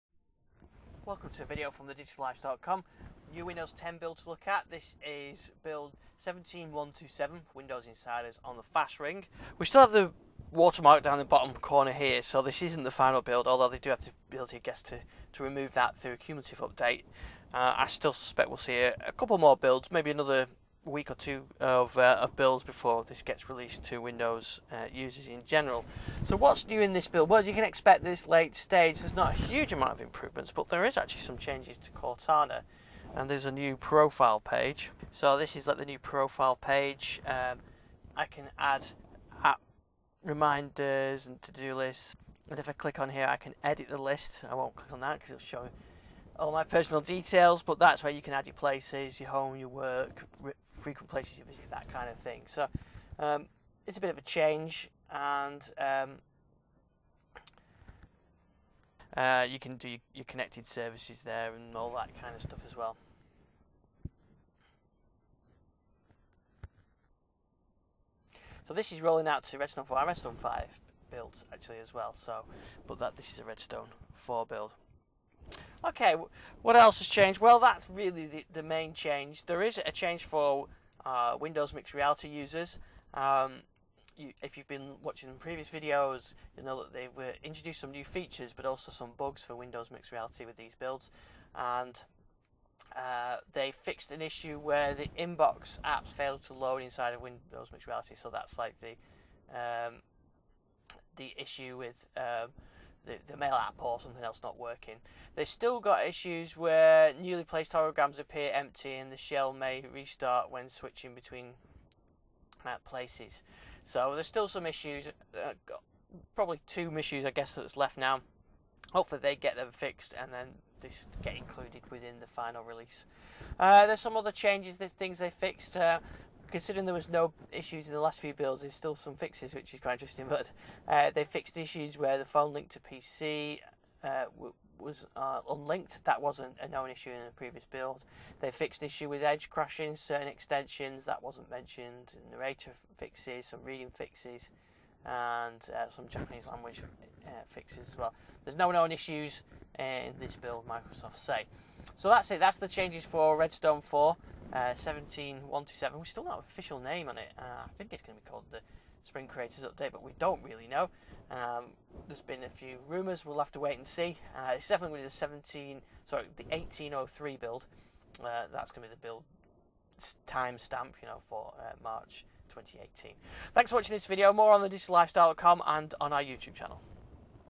Hands on with Windows 10 Redstone 4 build 17127
Welcome to my latest hands on video, this time looking at the changes in Windows 10 build 17127. This build has some changes to Cortana and fixes to Windows Mixed Reality. Not all the issues are fixed for Windows Mixed Reality but there are no other known issues.
In the video I look at the changes and the other fixes to the OS.